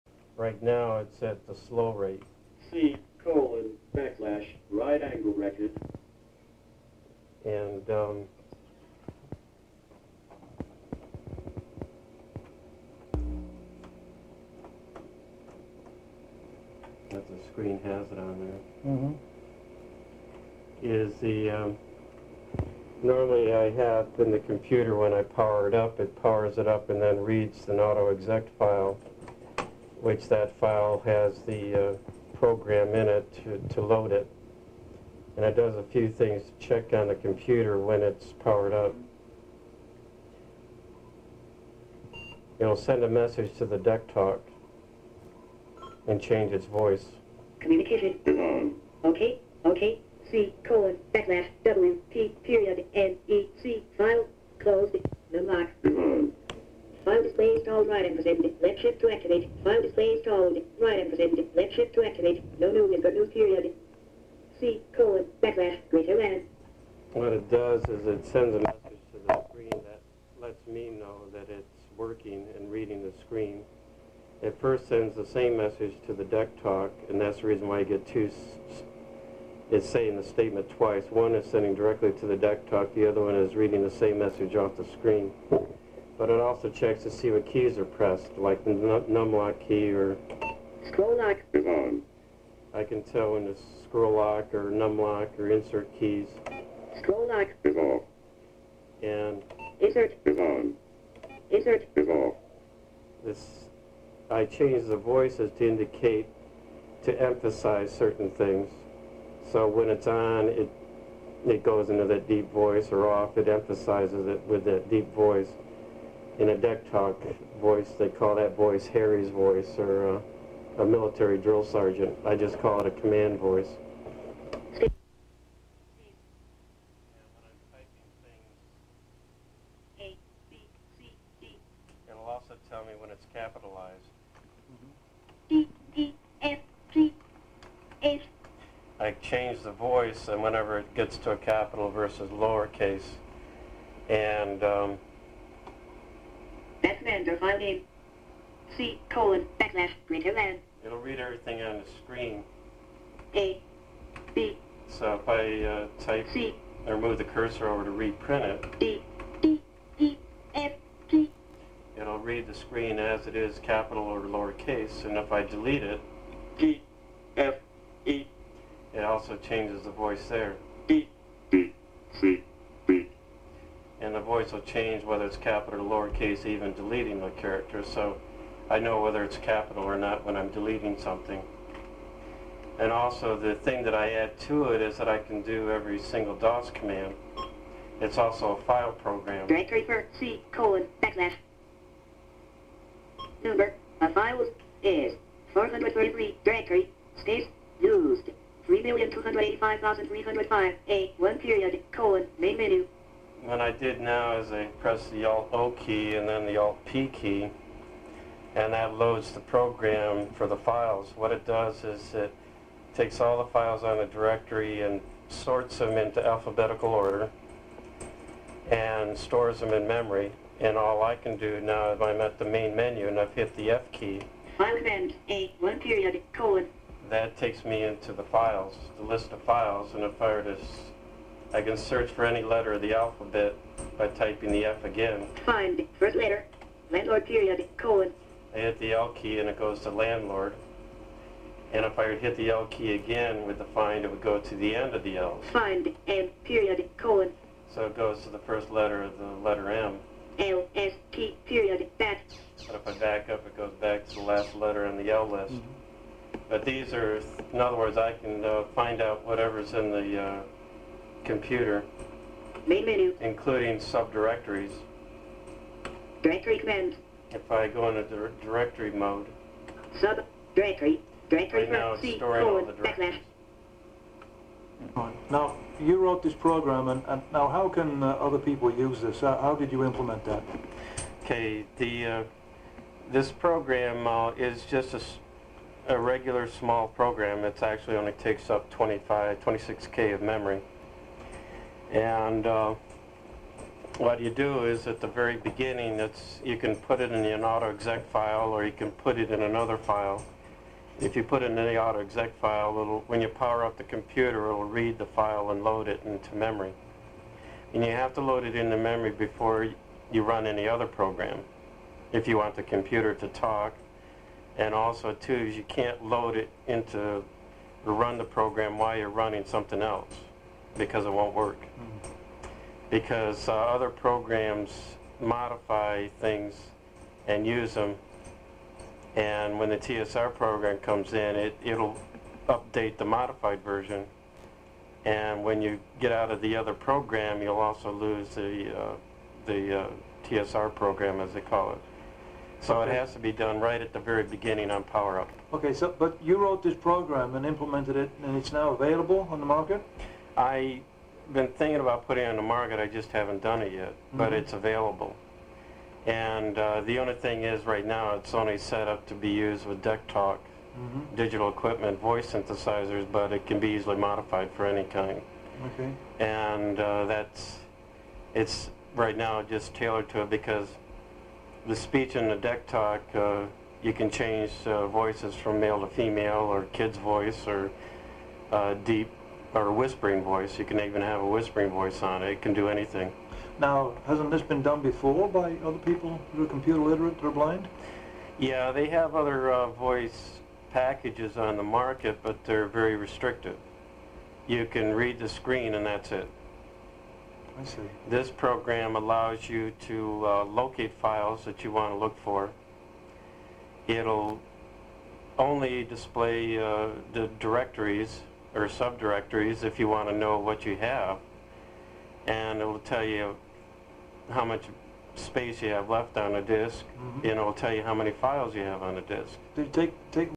Screen Reader Demo
Showing the ability of a visually impaired man who wrote his own screen reader program using just recorded tapes of one book and a one class course in assembly language.
Showing what is out there in computer generated speech.